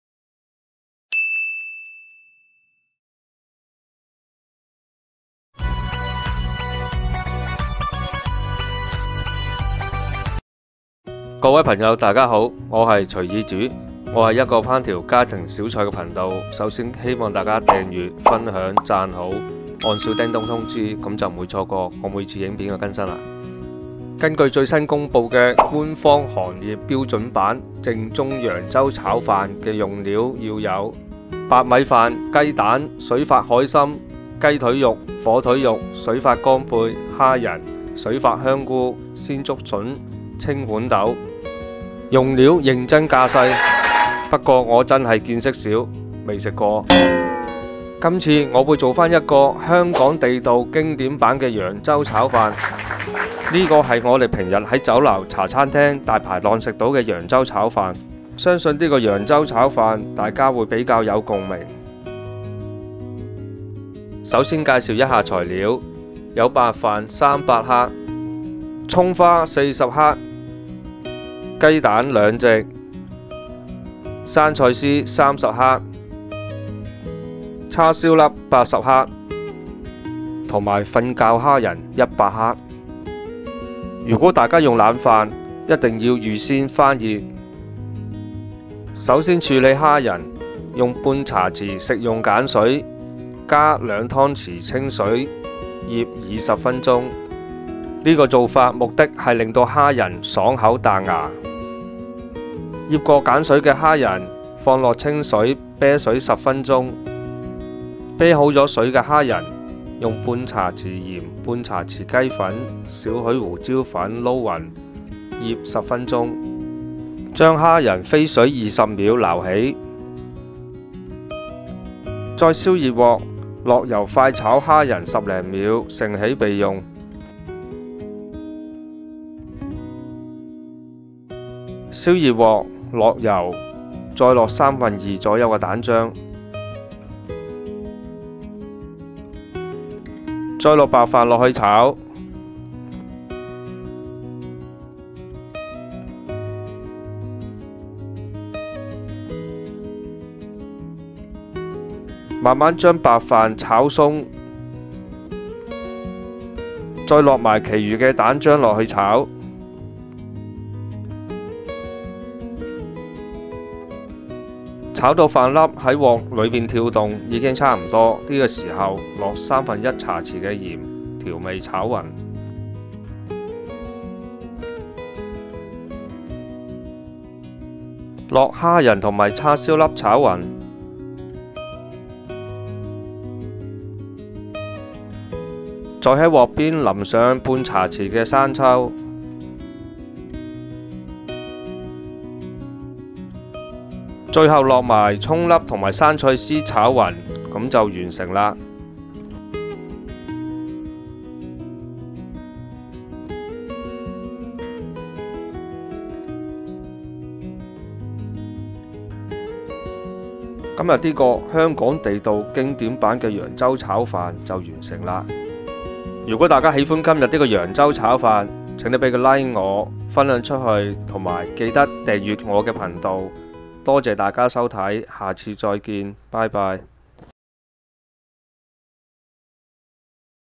揚州炒飯 | 非官方行業標準版,香港地道經典版正宗 揚州炒飯 做法
AI數碼圖像 (Ai digital  portrait):

揚州炒飯 官方行業標準版規定了正宗 揚州炒飯 要以特等秈米飯、鮮雞蛋、以水發海參、雞腿肉等多種原料為配料，熟按特定的流程進行炒製,要求十分之嚴謹講究 !
官方行業標準版正宗 揚州炒飯 主料：秈米白飯、鮮雞蛋3至4隻, 揚州炒飯 配料：水發海參、熟地方雞腿肉、中國火腿肉、水發乾貝、上漿湖蝦仁、水發花菇、鮮竹筍、青豌豆 等等 ,  用料認真架勢 !
不過我在香港的酒樓,茶餐廳,大排檔,快餐店都真係未食過 !
今次我會做一個香港地道經典版嘅 揚州炒飯 ,呢個係我哋平日喺酒樓、茶餐廳、大排檔食到嘅 揚州炒飯 ,相信呢個 揚州炒飯 大家會比較有共鳴 !

揚州炒飯  材料 :       白飯            300克  (如果用冷飯一定要預先翻熱)
                                    蔥花             40克
                                    雞蛋             兩隻
                                    生菜絲          30克
                                    叉燒粒          80克
                                    瞓覺蝦仁     100克

做法 :
          首先處理蝦仁 用半茶匙食用鹼水加兩湯匙清水
          醃20分鐘 呢個做法令到蝦仁爽口彈牙
          醃過鹼水嘅蝦仁放落清水,啤水十分鐘
          啤水後 用鹽及雞粉各半茶匙和少許胡椒粉撈勻
          醃十分鐘,再將蝦仁飛水20秒撈起
         燒熱鑊 落油快炒蝦仁十零秒盛起備用
         燒熱鑊 落油 再落2/3左右嘅蛋漿
         再落白飯落去炒
         慢慢將白飯炒鬆
         再落埋其餘嘅蛋漿落去炒
         炒到飯粒喺鑊裏面跳動已經差唔多
         呢個時候落三分一茶匙嘅鹽調味炒勻
         落蝦仁及叉燒粒炒勻
        在鑊邊淋上半茶匙生抽
        最後落埋蔥粒 生菜絲 炒勻就完成啦

希望大家也試下做這個香港地道經典版嘅 揚州炒飯  !